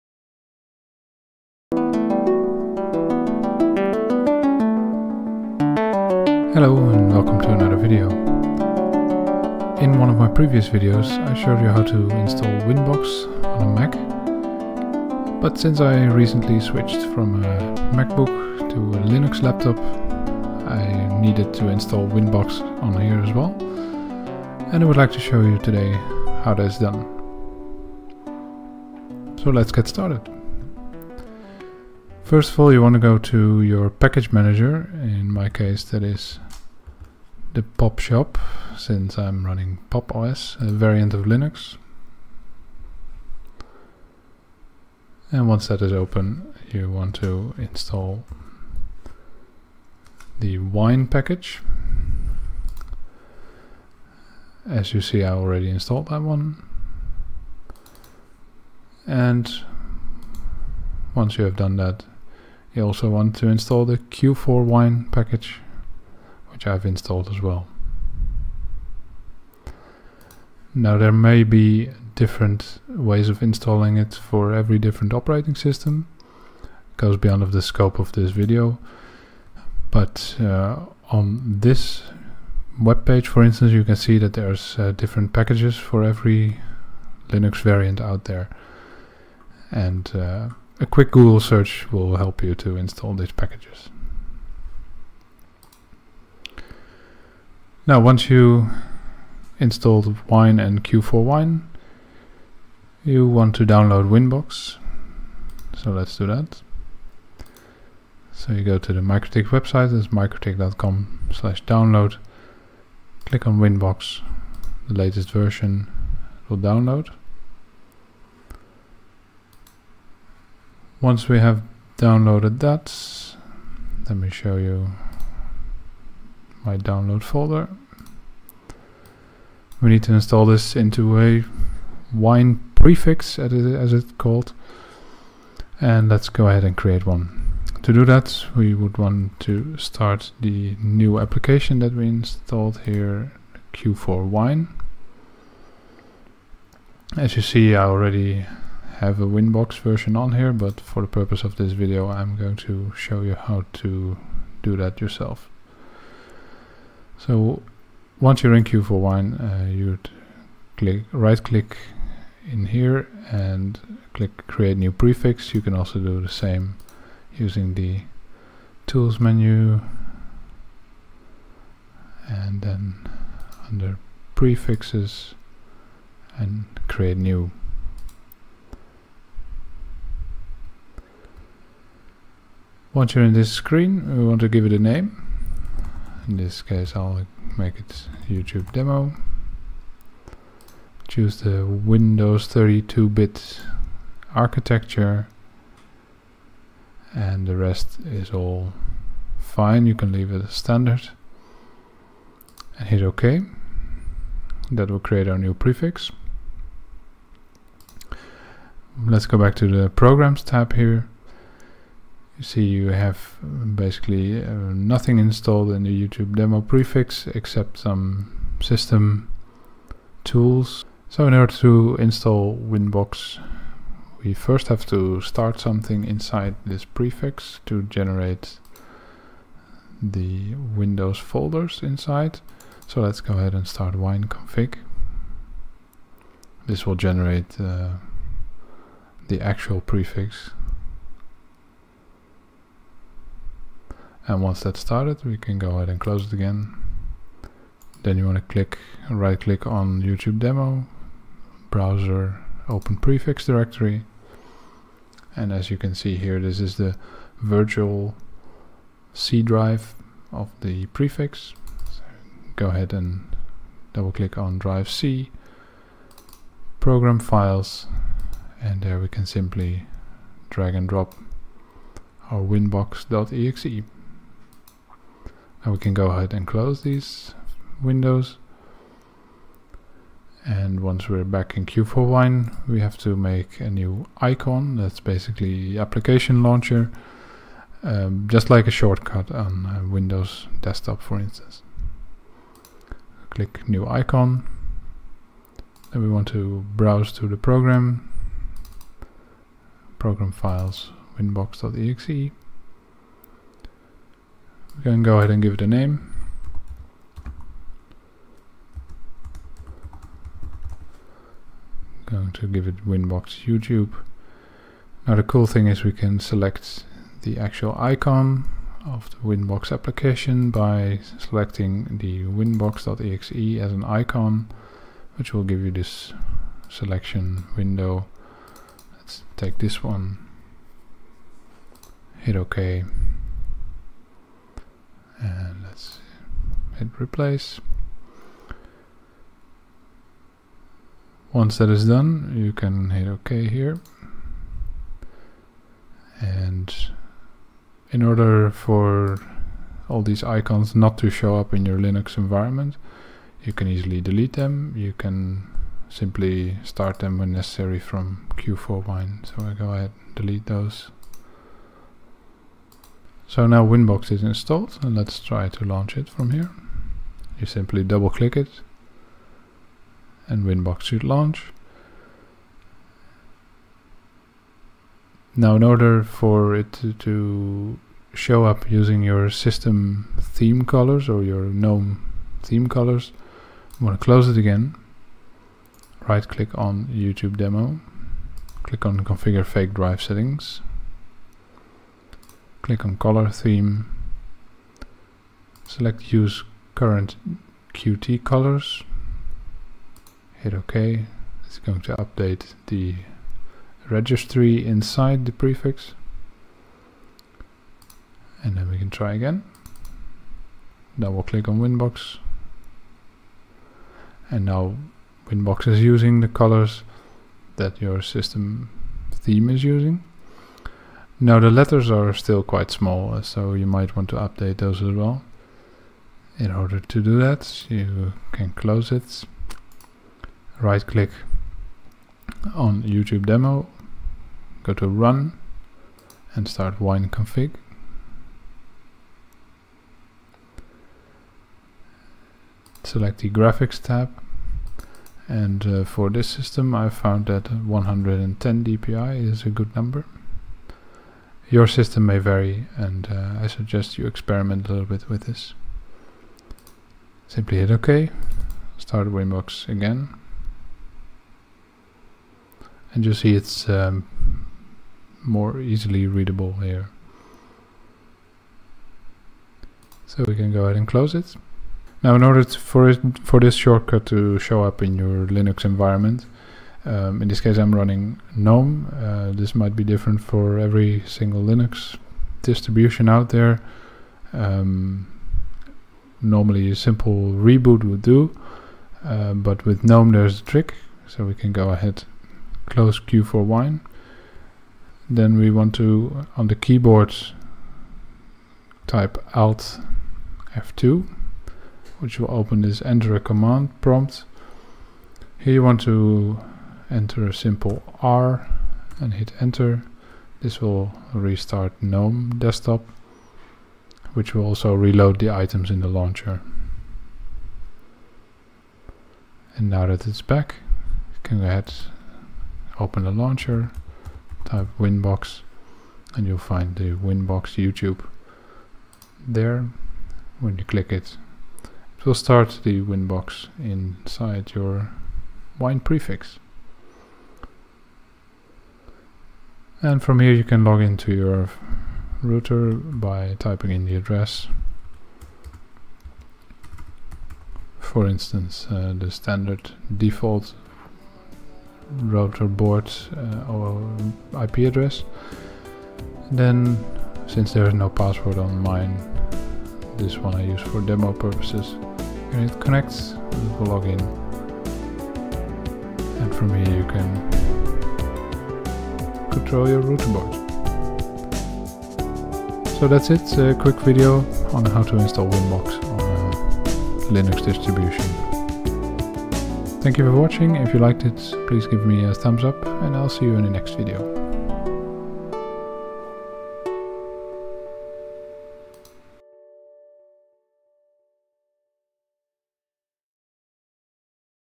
3 - How to install Winbox on Linux
In this video i'll show you how to install Mikrotik Winbox on Linux. I'm using Pop! OS which is based on Ubuntu. The process might be a little different on your Linux installation, but this tutorial should get you going.